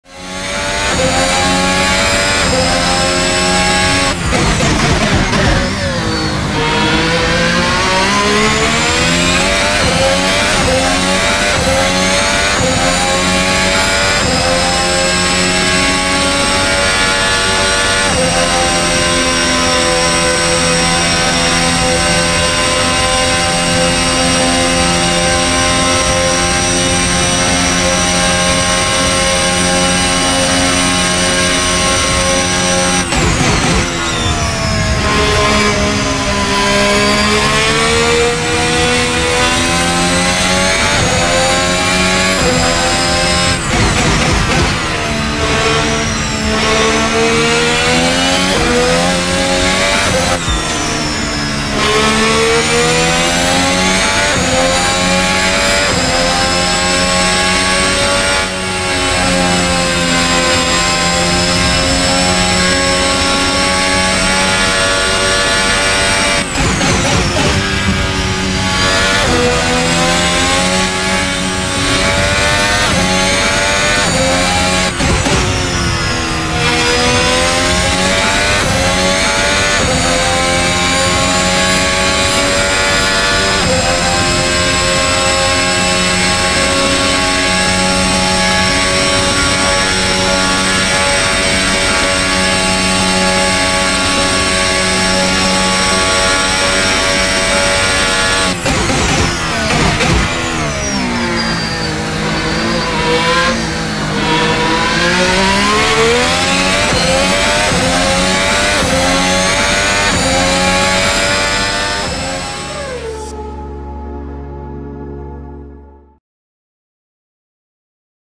Vuelta onboard: el circuito de Spa-Francorchamps, sede del GP de Bélgica de Fórmula 1
Nos subimos al Red Bull RB16B de Max Verstappen y Checo Pérez para descubrir cómo es el mítico trazado de Spa-Francorchamps, sede del Gran Premio de Bélgica de F1, desde dentro del cockpit.